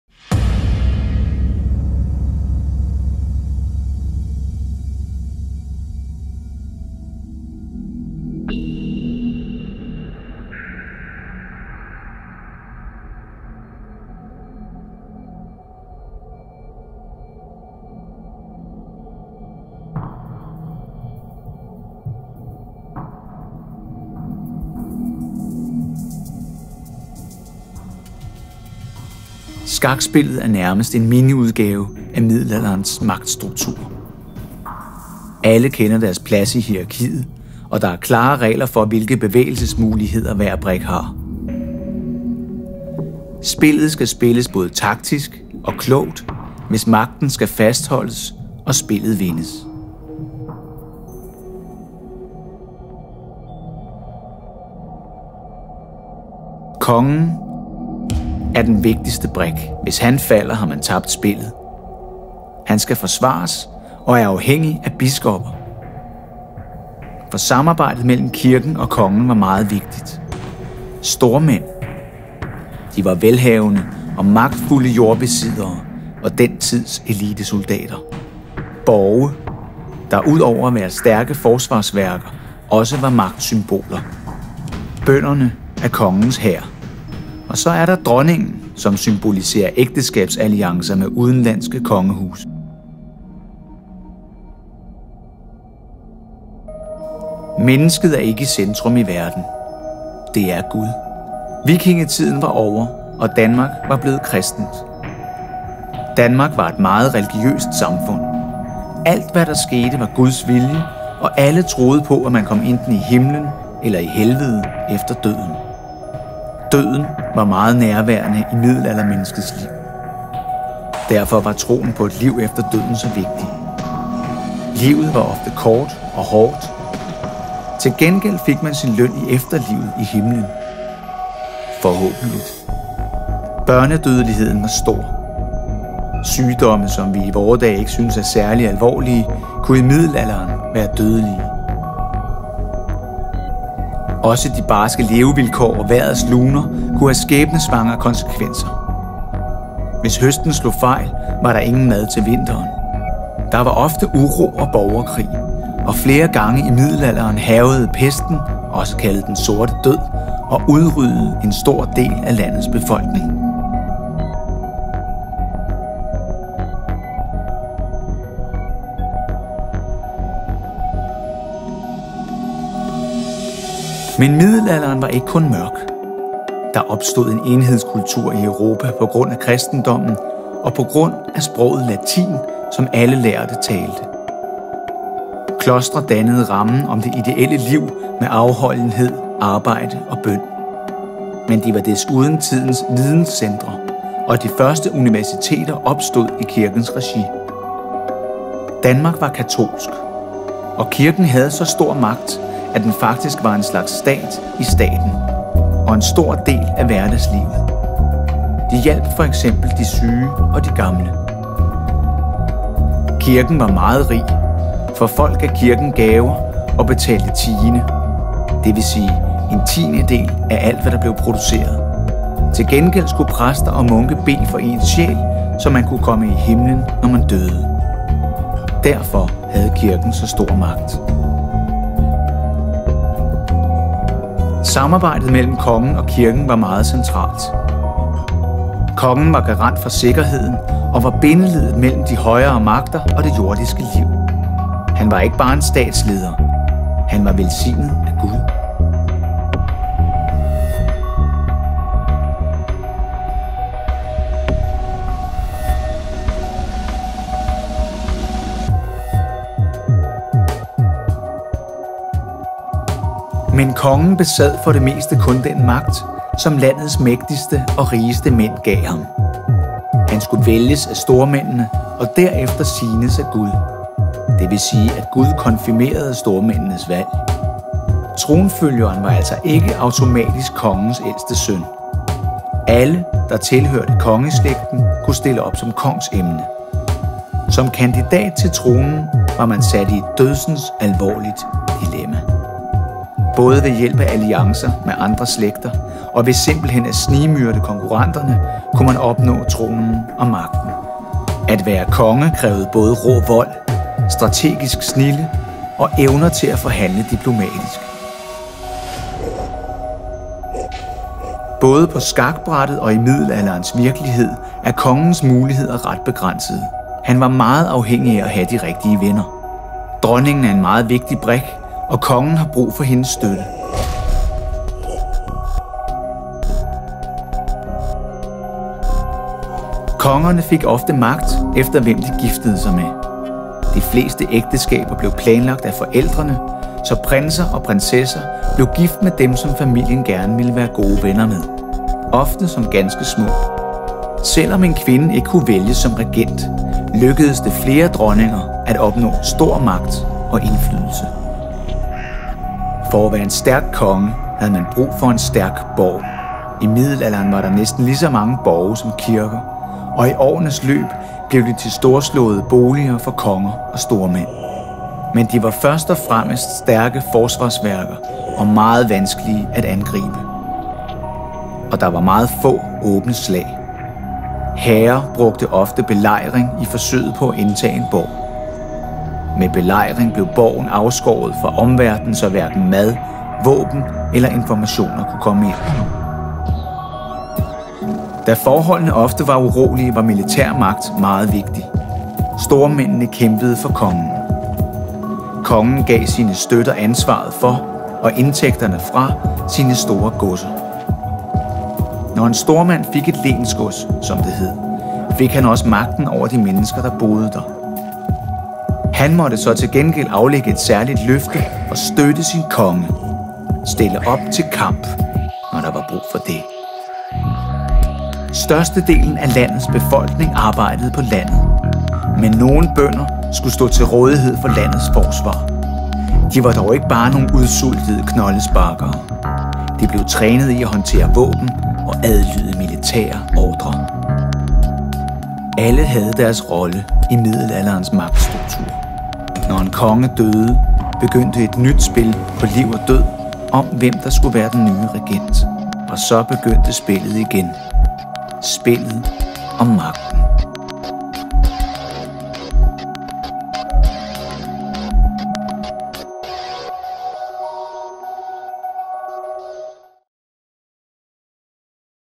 Middelalderens univers
Middelalderens magtspil var som et spil skak. Se instruktionsfilmen til Blueblood Academys undervisningstilbud.